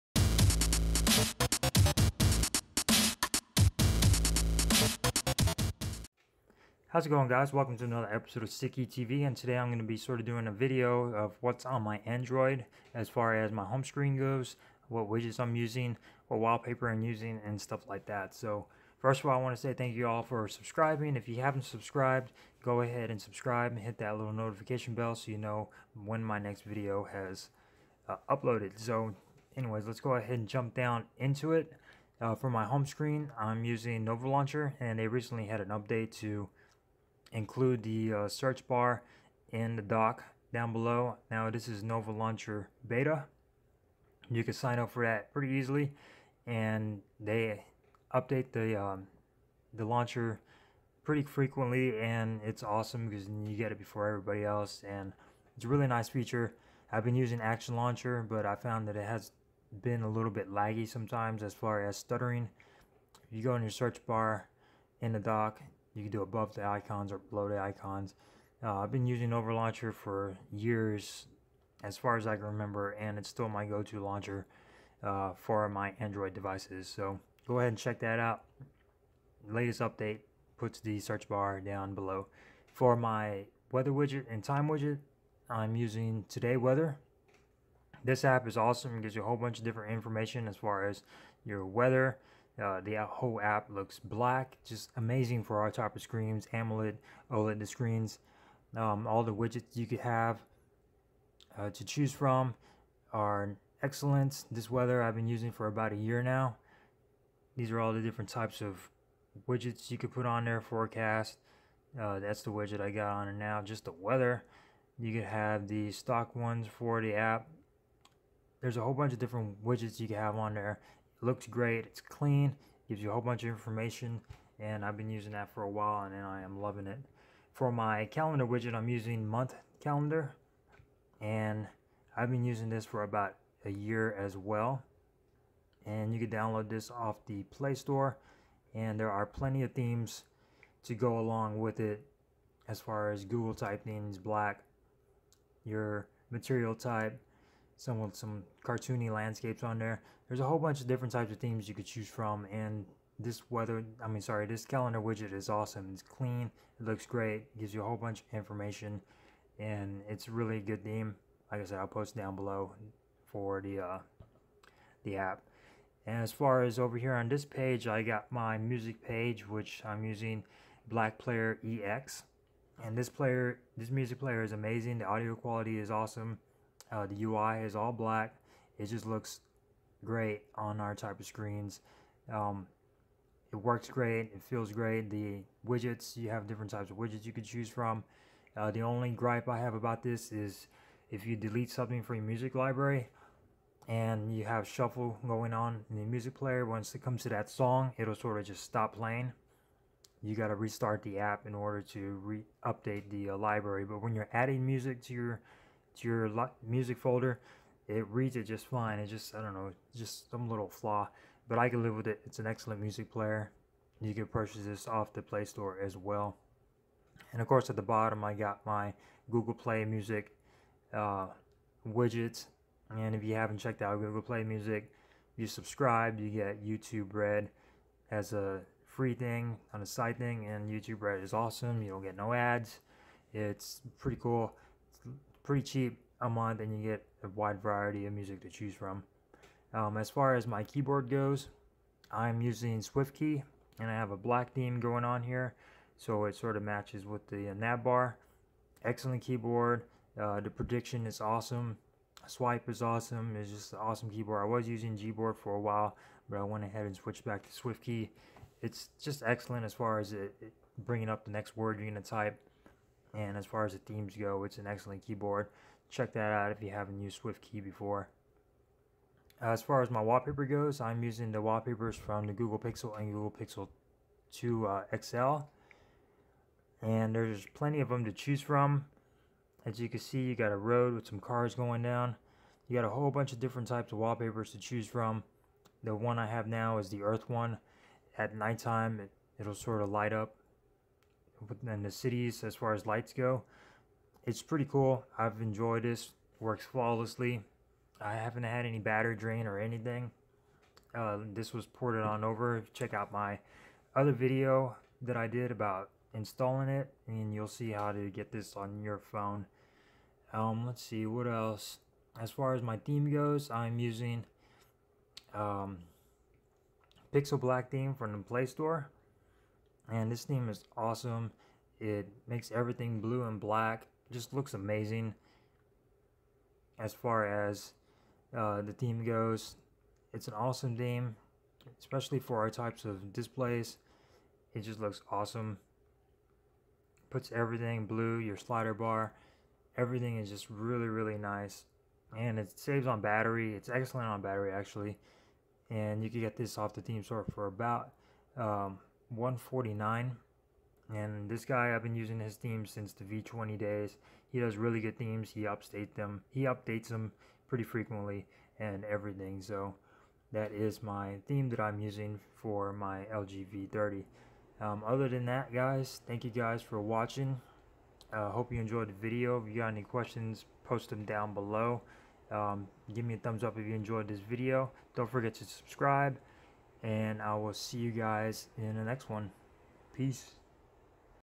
What's on my Android feat. LG V30
This is my first, What's on my Android video. Just going over home screen set-up and widgets, and etc.